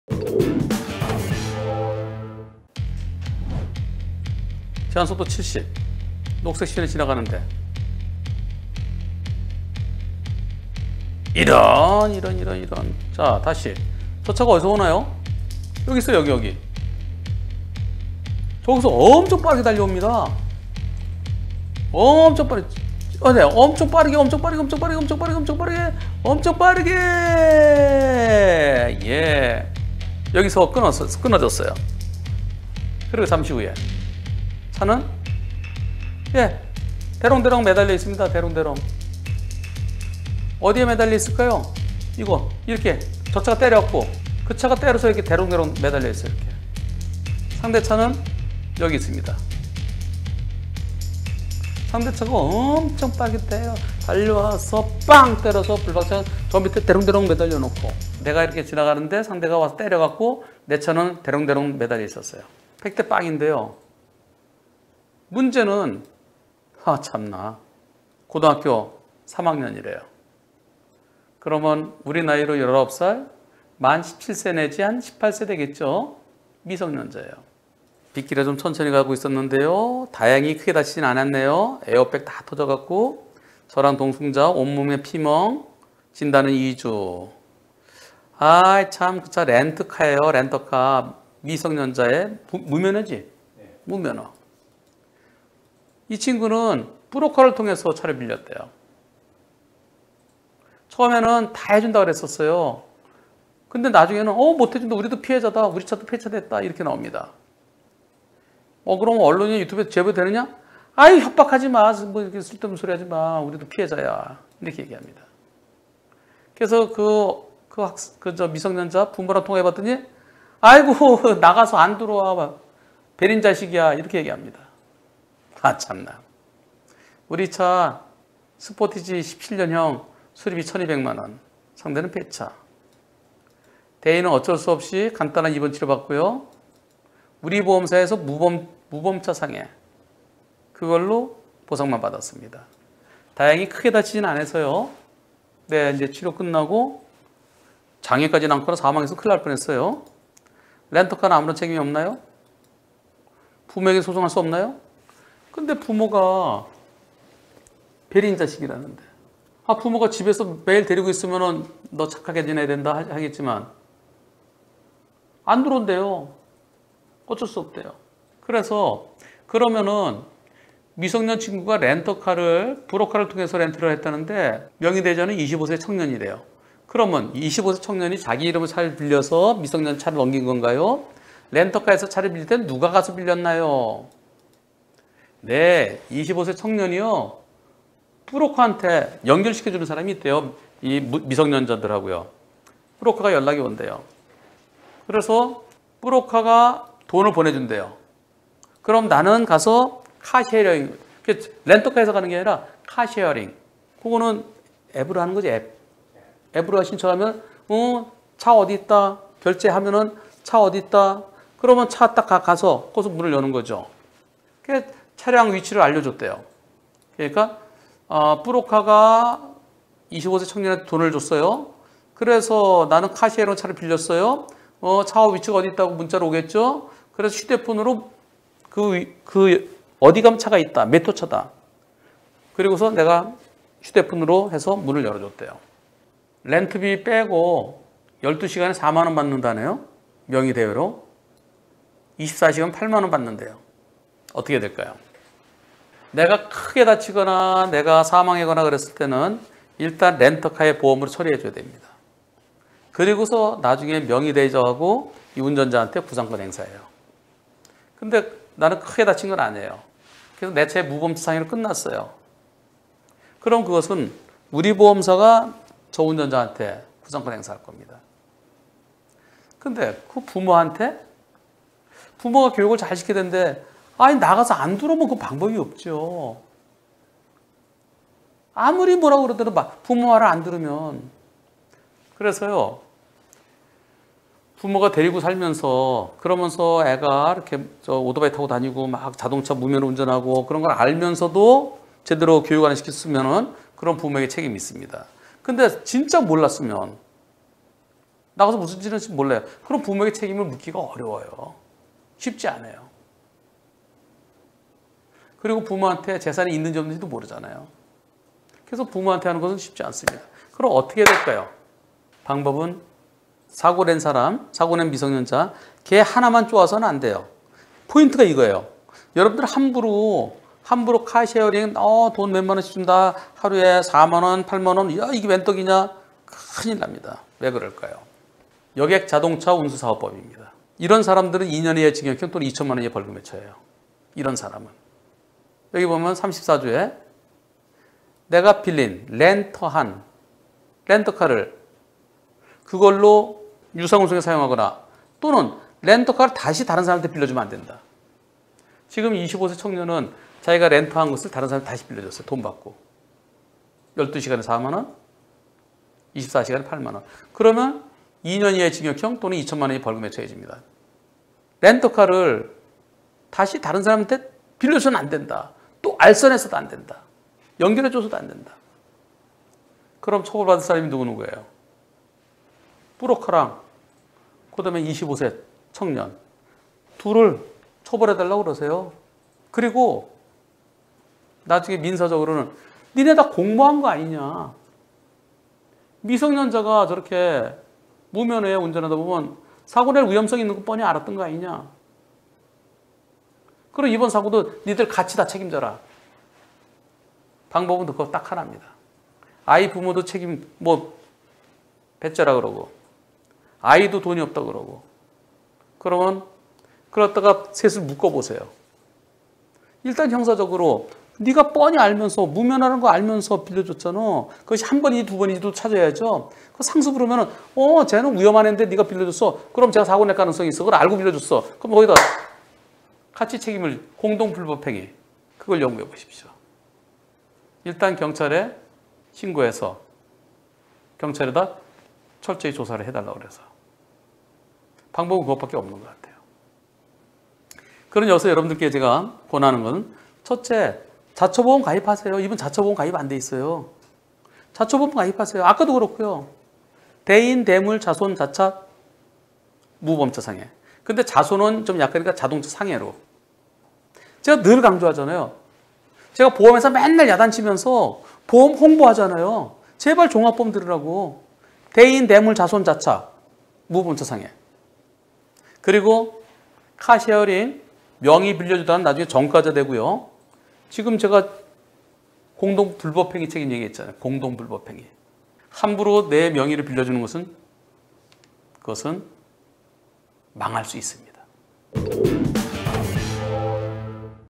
6963회. 고등학생이 운전하는 차와 사고가 났습니다. 그 차는 폐차했습니다. 무면허입니다. 부모가 책임을 못 진답니다. 카 쉐어링인데 불법 대여네요. 난감합니다
(b9570, 무면허 미성년자 불법 렌터카 난감합니다

교차로를 통과하는데 오른쪽에서 엄청 빠르게 달려온 차와 사고.
충돌 후 블박차는 논두렁에 처박힘. 당연히 100:0 인 사고. 그런데...

상대 운전자는 18세 미성년자에 무면허 . 렌터카

1. 그 도로 제한속도는 시속 얼마이고 블박차 속도는 얼마였나요?
     →  70키로 국도에 저희는 빗길이라 50키로 서행 중이였습니다

2. 이 사고로 다친 사람 있나요?
     → (누가 어딜 얼마나 다쳐 몇 주 진단인가요?)
           운전자 동승자 2인이 탑승하였고
           온몸에 피멍이 들었습니다
           타박상 염좌 뇌진탕등으로 2주 진단받았습니다

안녕하세요 변호사님
저에게도 이런 어이없는 일이 일어나네요.
혼자서 일처리가 힘들어서 제보하게됩니다.

상대 차량은  렌트 차량이며 미성년자가 운전을했고요.

제가 알아본 바 이 미성년자 친구는 렌트카 대여 브로커를 통해 렌트를 했으며 명의 대여자는 25세 청년으로 알고 있습니다.

렌트카측에서는 처음에는 당연히 본인차량이 가해차량이라 보상을 해준다 해놓고
지금은 '운전자 또는 대여자랑 보상 협의를 봐라,
우리 렌트카측은 회사 차량도 폐차다. 우리도 피해자다, 아무런 책임이 없다'고  하는 상태입니다.

언론이나 유튜브 제보해도 되냐니까
같지도 않은 협박하지 말고 제보하랍니다;;

형사분께서 부모랑 통화 시도했지만
부모 또한 모른 척 한답니다. 알아서 하라고...

저희 차량 스포티지 17년형 수리비는 1200만원이고요. 상대차량은 폐차랍니다.

대인은 어쩔 수 없이 간단한 입원치료만 받고
저희 보험사한테 무보험 특약으로 보상만 받은 상태이고 저희 자차는 가입되지 않아 보상이 막막합니다.

질문1. 렌트카는 아무런 책임이 없나요??
질문2. 부모에게 소송가면 피해 금액 100프로 가능한 지

* 한문철 변호사
이 미성년자 친구는 렌트카 대여 브로커를 통해 렌트를 했으며
명의 대여자는 25세 청년으로 알고 있습니다.

==)  25세 청년이 자기 이름으로 차를 빌려온 후
미성년자에게 차를 넘긴건가요?

렌터카 회사에서 차를 빌릴 땐
누가 가서 빌렸나요?

ckw
네 25세 청년이 브로커에게 입금을 받고
카쉐어링으로 차량을 대여한 차량위치 알려 준 후 휴대폰으로 문을 열어줬다고 합니다.

 댓가는 차량 대여비 제외 12시간 4만원, 24시간 8만원을 수익으로 받고 명의대여를 해줬다고 합니다.

내가 크게 다치거나 사망했다면 일단 렌터카 보험으로 처리하고 나중에 운전자와 명의대여자에게 구상.
내가 크게 다치지 않아서 내 보험(무보험차 상해)으로 처리하고 내 보험사가 운전자에게 구상권 행사할 것.

그런데, 부모에게 책임 묻기도 쉽지 않음(부모가 내놓은 자식)
운전자는 자차도 없음

브로커와 25세 청년을 처벌해달라고하고. 나중에 민사적으로, 사고낼 위험성을 뻔히 알면서 한 행위에 대해 사고를 같이 책임지게 함. 공동불법행위

포인트-
1. 자차 보험 가입하세요!! (대인 대물 자손 자차 무보험차 상해)
2. 함부로 카 쉐어링 명의 빌려주면 전과자됩니다

 여객자동차 운수사업법

 제90조(벌칙) 다음 각 호의 어느 하나에 해당하는 자는 2년 이하의 징역 또는 2천만원 이하의 벌금에 처한다.

4. 제28조제1항에 따른 등록을 하지 아니하고 자동차대여사업을 경영한 자

6의2. 제34조제1항을 위반하여 임차한 자동차를 유상 운송에 사용하거나 다시 남에게 대여한 자 또는 이를 알선한 자

제34조(유상운송의 금지 등)

① 자동차대여사업자의 사업용 자동차를 임차한 자는 그 자동차를 유상(有償)으로 운송에 사용하거나 다시 남에게 대여하여서는 아니 되며, 누구든지 이를 알선(斡旋)하여서는 아니 된다.

   ② 누구든지 자동차대여사업자의 사업용 자동차를 임차한 자에게 운전자를 알선하여서는 아니 된다. 다만, 외국인이나 장애인 등 대통령령으로 정하는 경우에는 운전자를 알선할 수 있다.